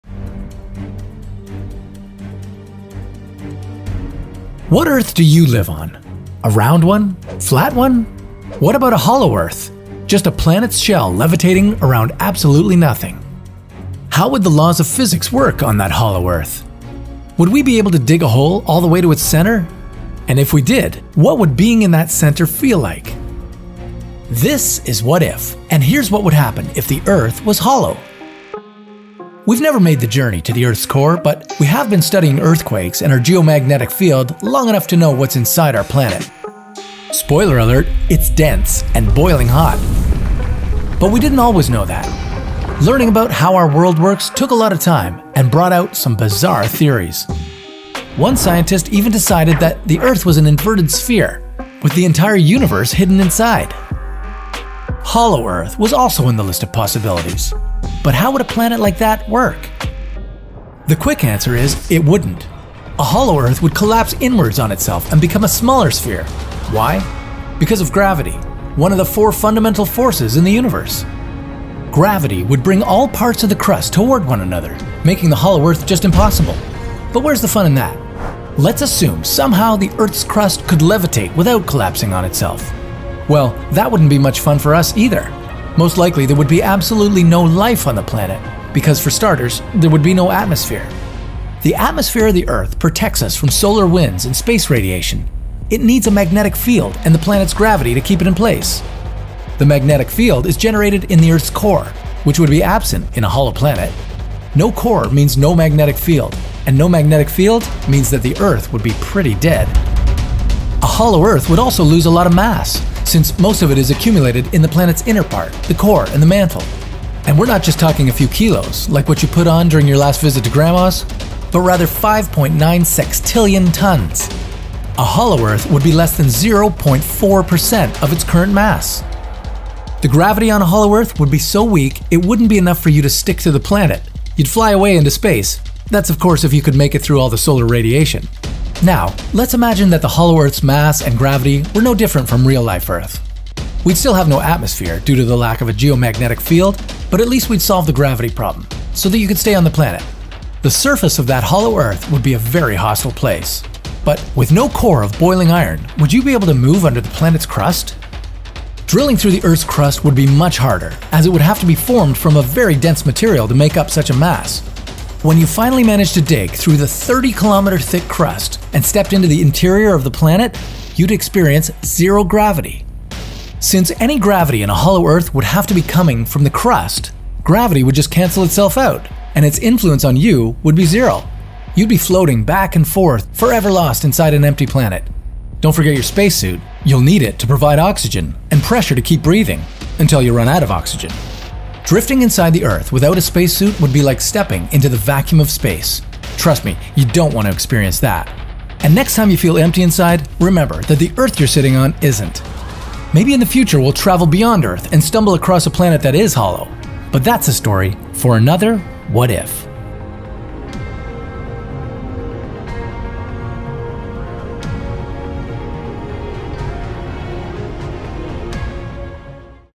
만약에 지구가 텅 빈다면? | 만약에 What If Korea
우리는 어떤 지구에 살고 있을까요?
둥근 지구? 납작한 지구? 아니면 거대한 우주 거북이 등껍질에 살고 있는 건가요?
그러면 만약에 지구가 텅 빈다면 어떤 일이 펼쳐질까요?
물리학의 법칙들은 어떻게 작용될까요? 우리는 또한 살아남을 수 있을까요?